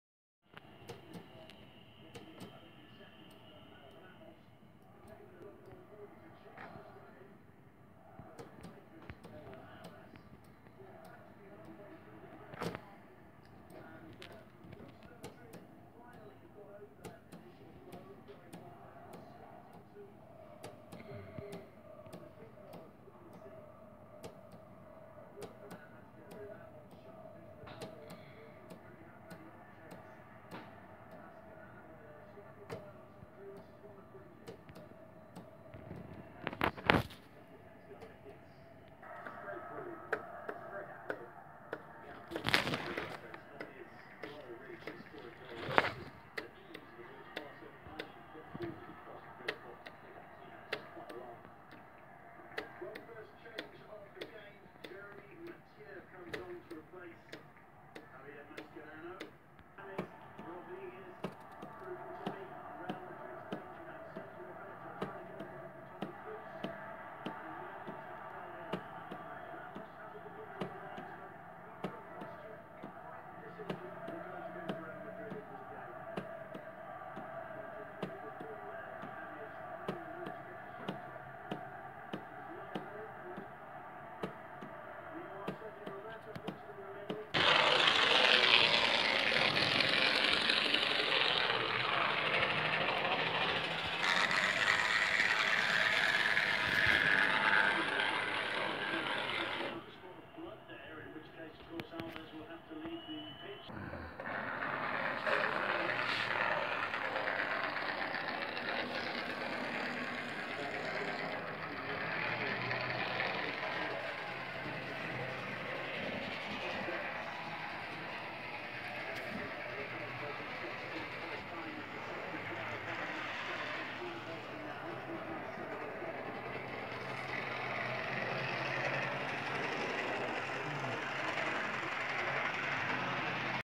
Simulasi anjlok kereta plasseur mtt di jembatan seri 08-16 GS skala HO 1/87
kereta ini handmade dari mang irin dan dalam keadaan penyelesaian / finishing 90% mencoba mengoprasikan tamping otomatis